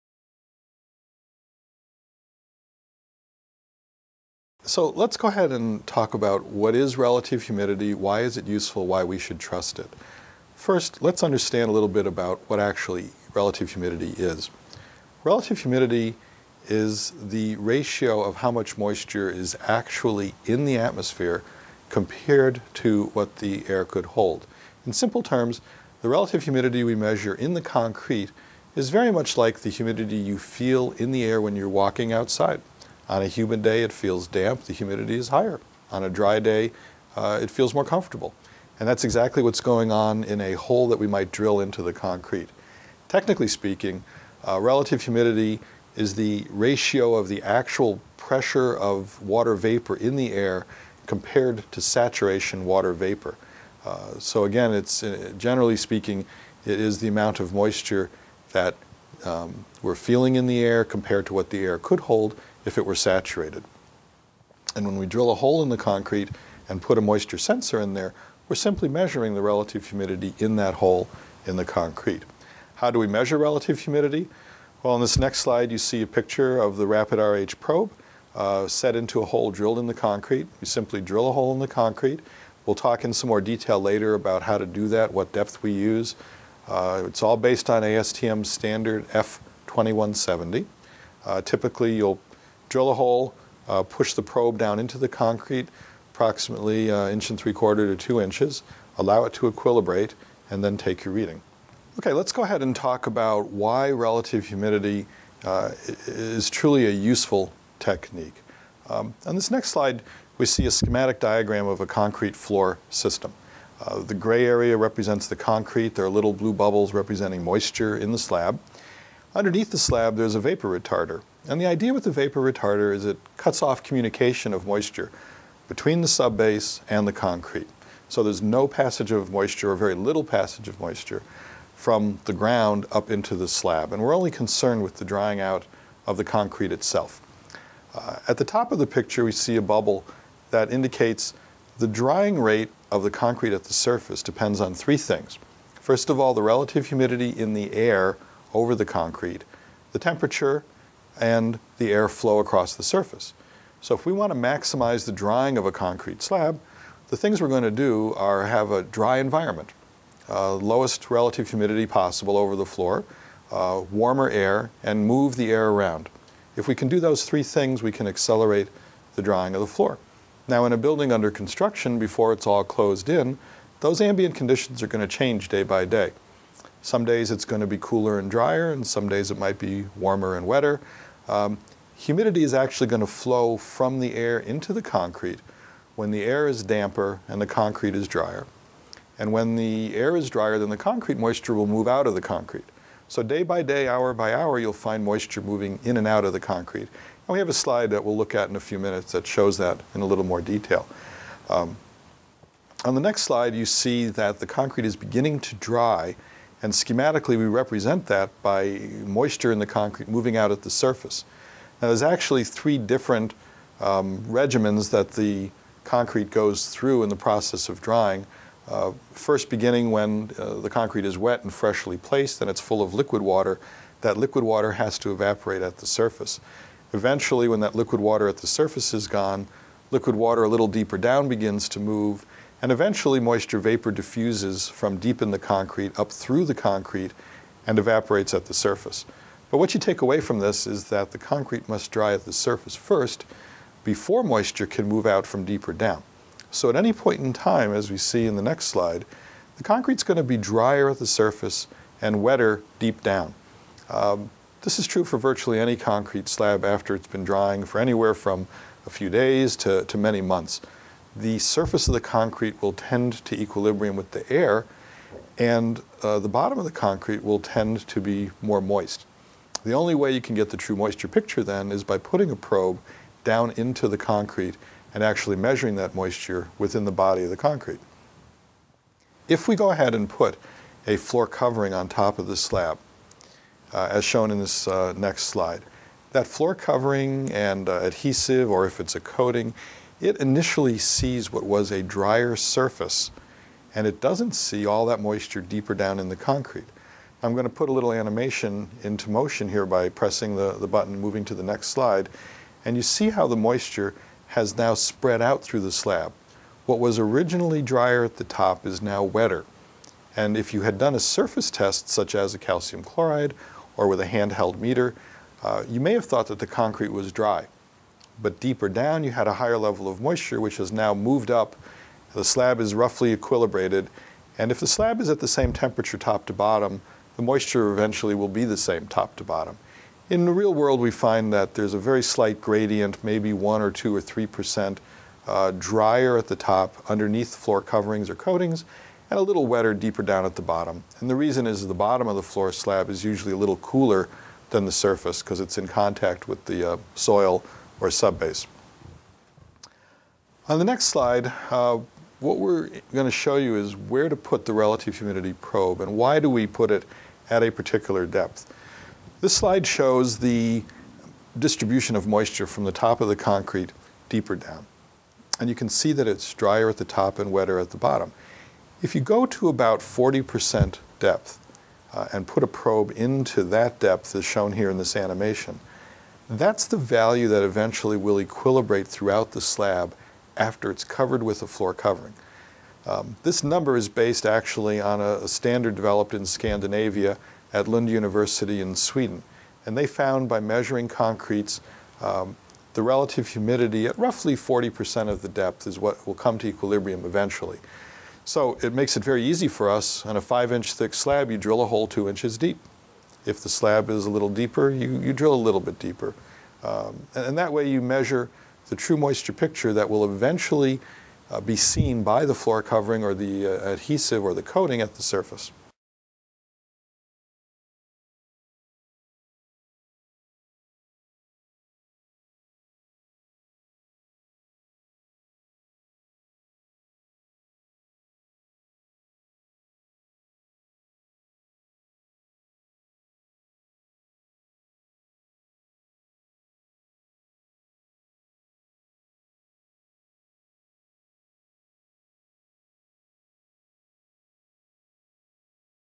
Relative Humidity the Most Useful Test Method - RH 8 of 21
(Source As a concrete slab dries, moisture moves in and out of the top of the slab. Kanare will show how measuring the RH at a deeper depth will reveal a true picture of the future moisture condition of the slab.